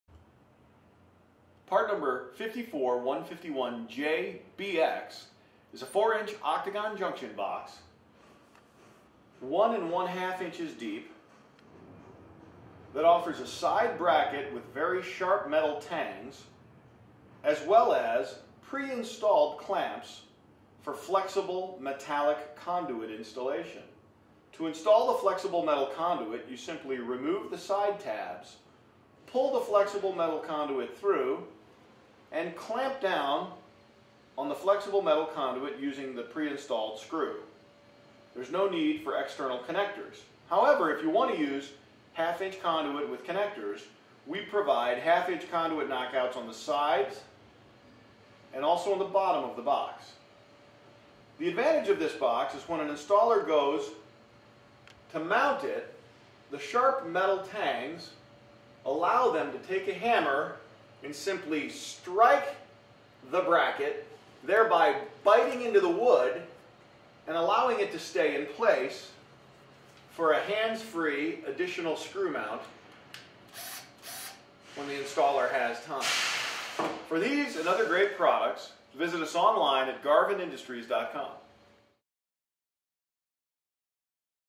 Octagon Box: 4" Octagon Box w/ Flush Mount Bracket & Clamps for Flexible Metal Conduit 54151-JBX
- 1-1/2" deep, 4" octagon boxes with flush mount brackets and clamps for flexible metal conduits are installed in walls and ceilings for lighting fixtures and other outlet devices that are powered using flexible metal conduits. Garvin Industries' cable clamps save both time and money by allowing the flexible metal conduits to be easily terminated in boxes without the need for costly external box connectors. The various mounting brackets allow easy box positioning along studs or joists.